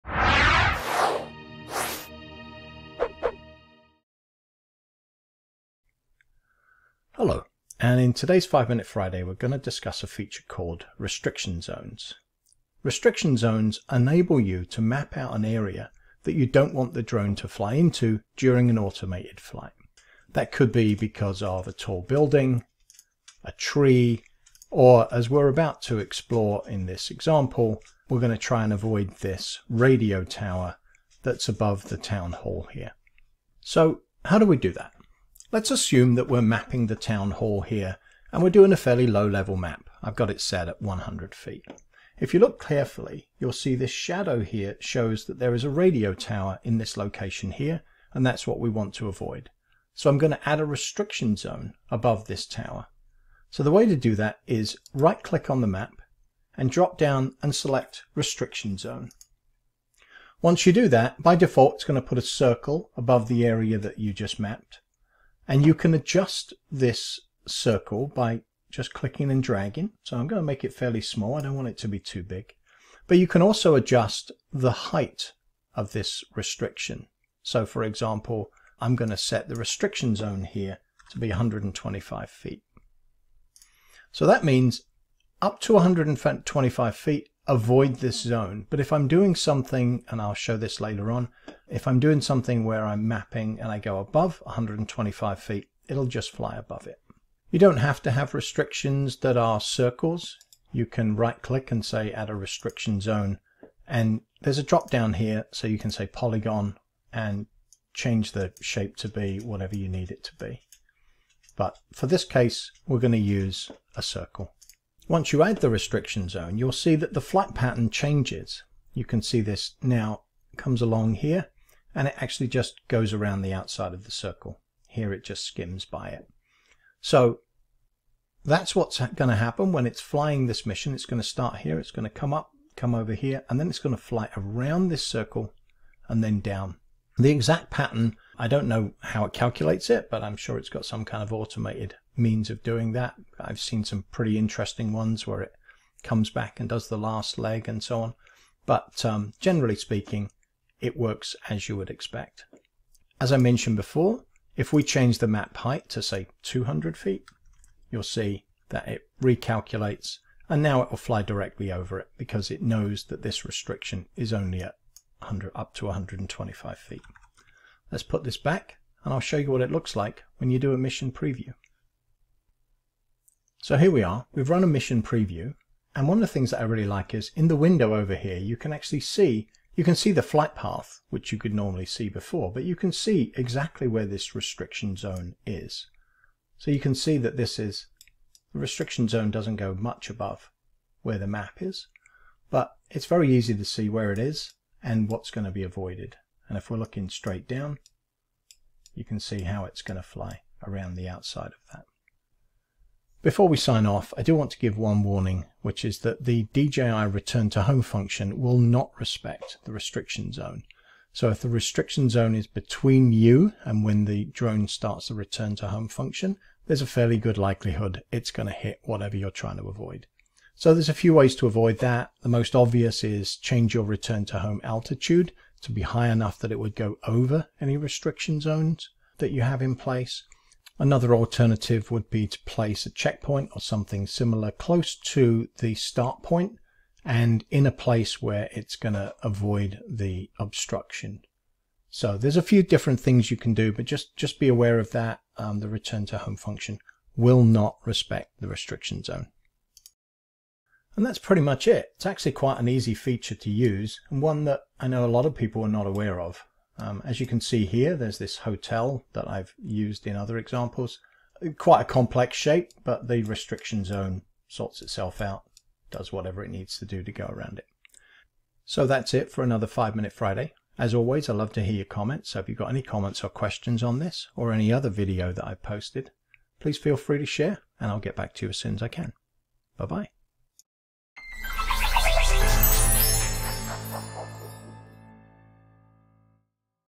Dronelink Restriction Zones
Do you have obstacles in the flight path that you need to avoid?  Restriction zones will take care of that quickly and easily.

*** Note that you must be in expert mode for the restriction zone option to show up ***

Avios Media offers drone automation consulting, and video production for Commercial Real Estate (CRE) and Construction Projects